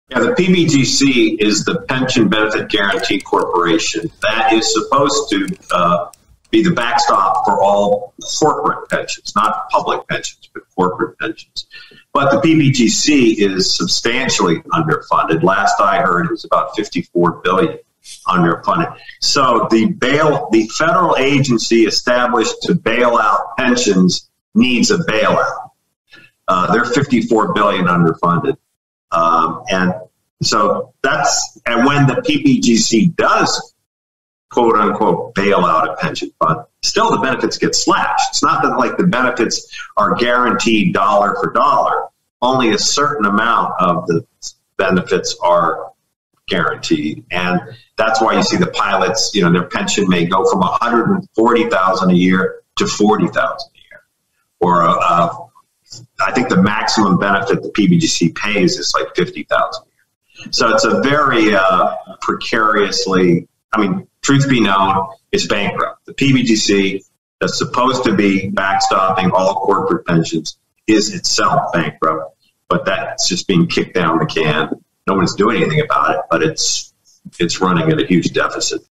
The Downfall Of The Pension Benefit Guarantee Corporation (PBGC) - Robert Kiyosaki
Retirement fund giant, CALPERS, voted in early November to use borrowed money and alternative assets to meet its investment-return target, even after lowering that target just a few months ago.

Today’s guests, John MacGregor and Ted Siedle discuss what this means for retirees depending on their pension for their financial future.